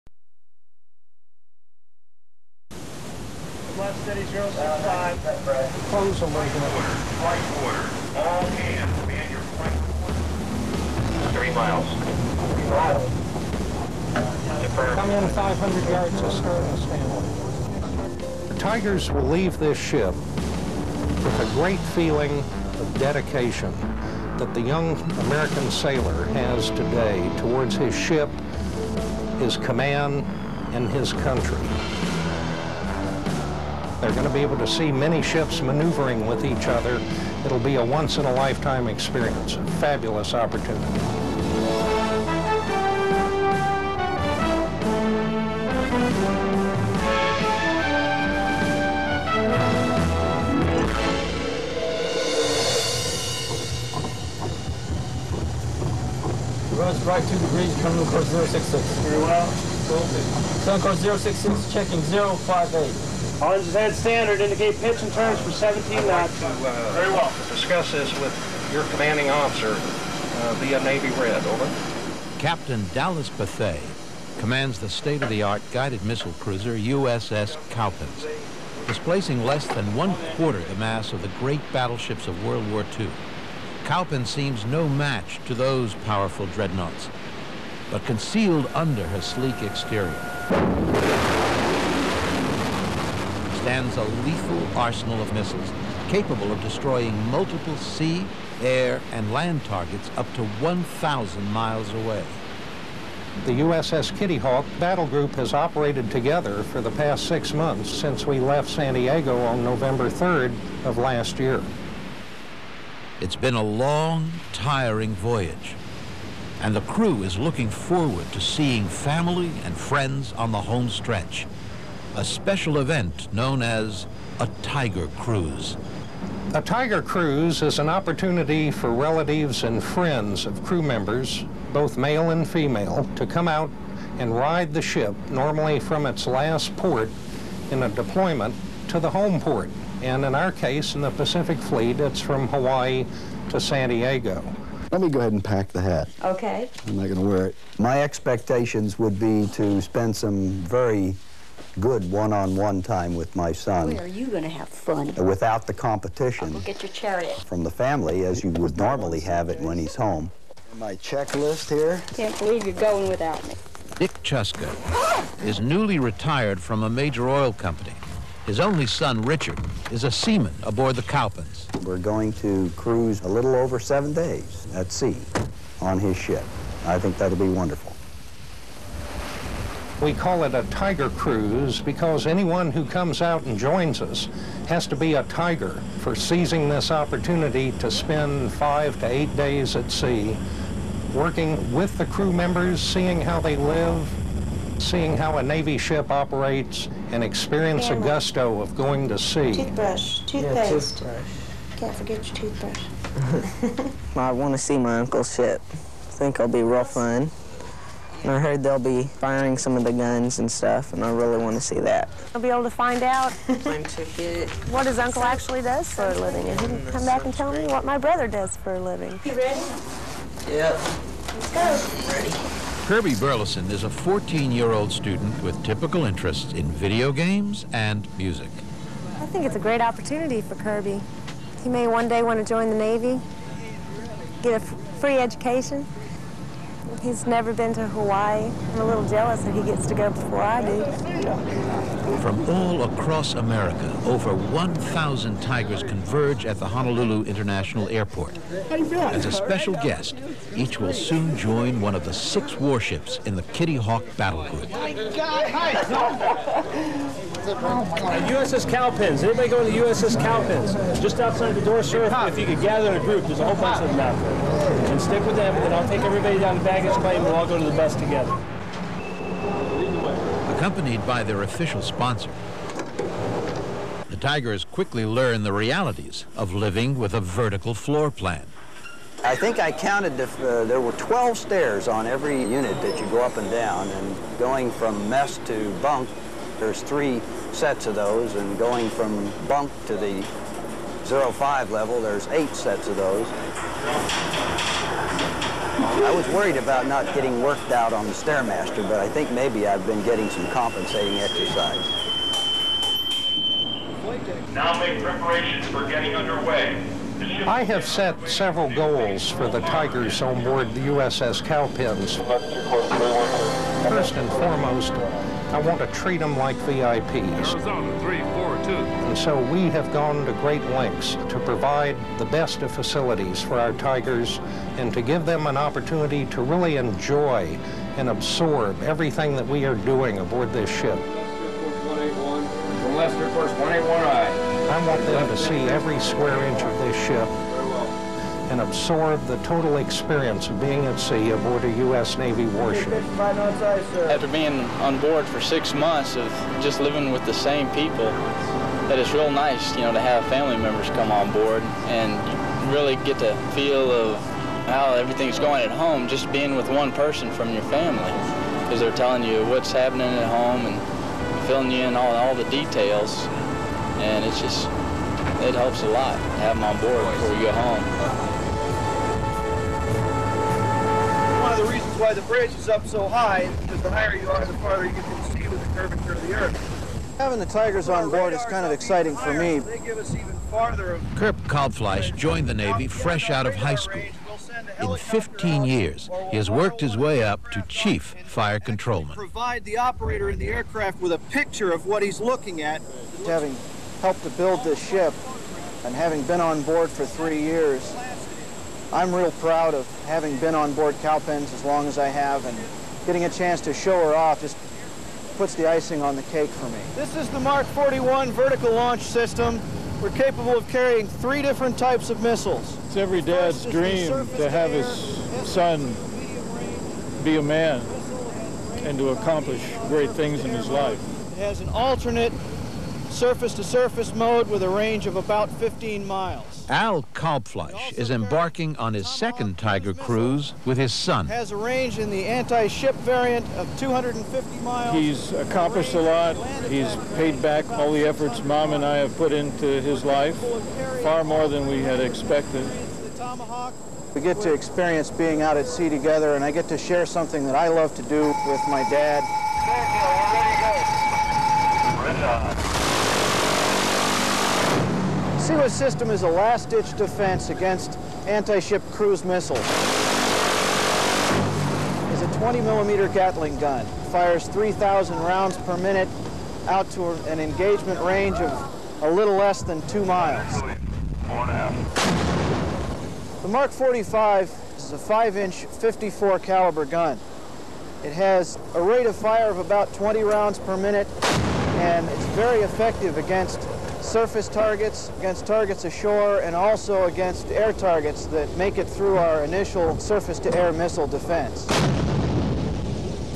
USS Cowpens (CG 63) Tiger Cruise - '93 (Part 1 of 3)
Part 1 of 3. As the USS Cowpens (CG 63) returns from the Persian Gulf, family of crew members join the ship at Pearl Harbor and ride back to San Diego, taking part in ship activities and learning about day-to-day life aboard a U.S. Navy ship. Interviews with command, crew, and guest ship-riders and footage of ship operations and capabilities.